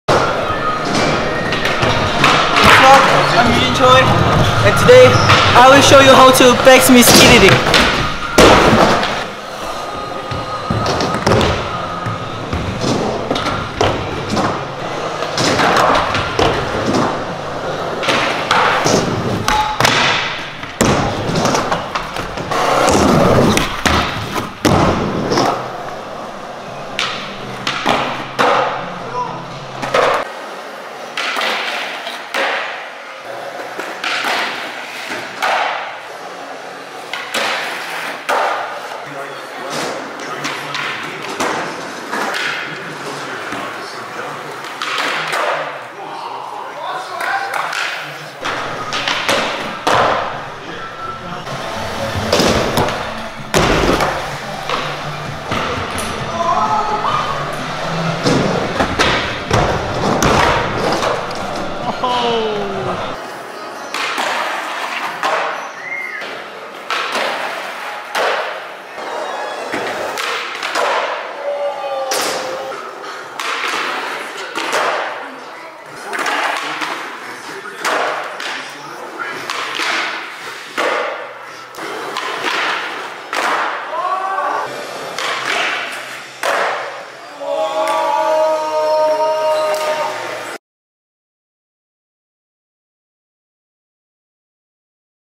LOST TAPES #5 | Crailtap Park [데일리 그라인드 스케이트보드 매거진]
2020년, Girl과 Chocolate Skateboards의 모회사인 Crailtap을 방문했습니다.

Featuring: Eugene Choi, Juwon Eun, Kwanghoon Cho, Katsuya Nakazawa

Filmed by Kwanghoon Cho, Koya Uno